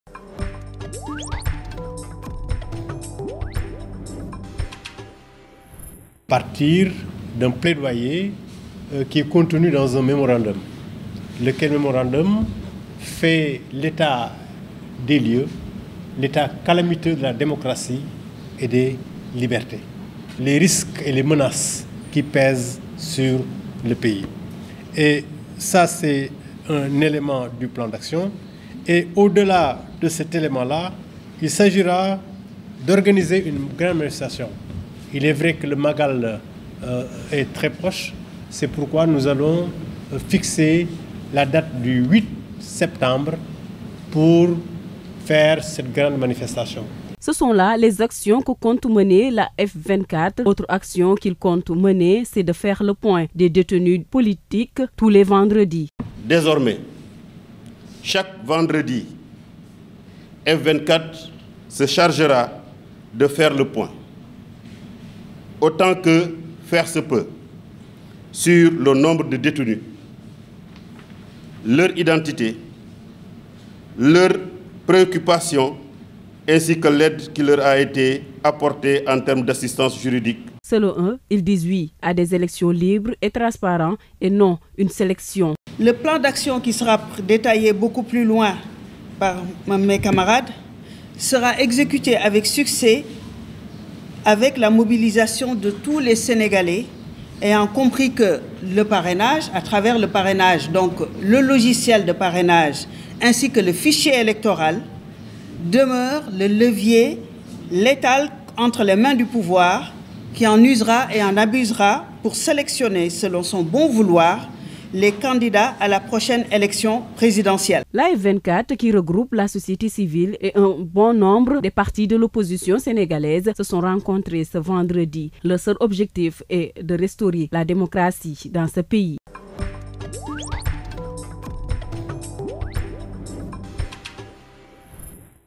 Pour la libération des détenus politiques : F24 révèle un plan d'action de quatre points
Un plan d'action, reposant sur quatre points a été révélé par la F24 et pour la libération des détenus politiques. Chaque vendredi, F24 se chargera de faire le point sur les détenus. Entre autres revendications, la structure veut des élections libres avec la participation de tous. Ils n'ont pas manqué d'interpeller les Sénégalais à les rejoindre dans ce combat pour la liberté et répondre présent au rassemblement qu'il compte tenir le 8 septembre prochain.